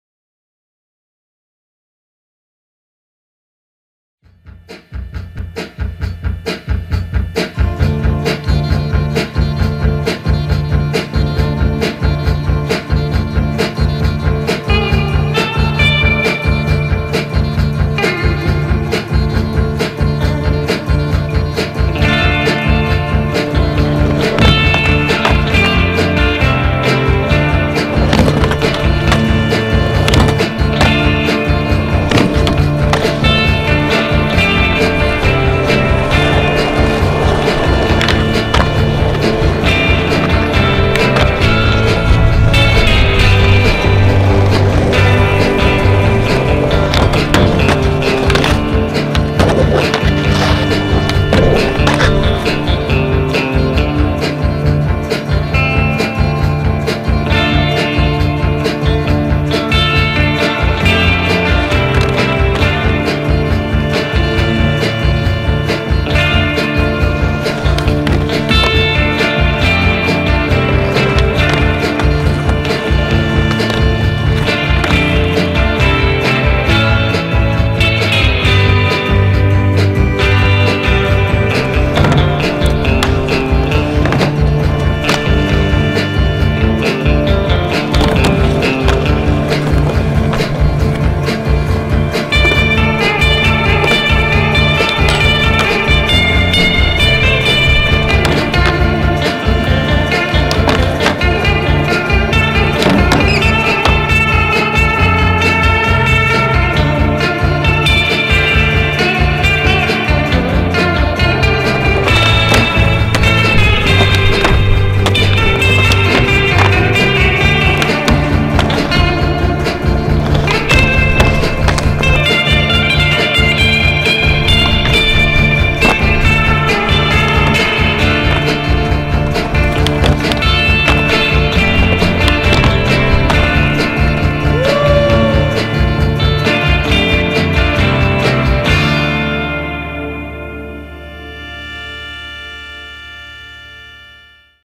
Taller de Rampas Consproject IV
Una nueva versión del clásico Taller de Rampas del Consproject benefició a nuevos y diversos spots de Santiago. Felipe Lavín y Amaru Maturana acompañaron la entrega de los obstáculos y aprovecharon de sesionar con los locales de cada spot.

Flimado y editado por Nicolás Garay.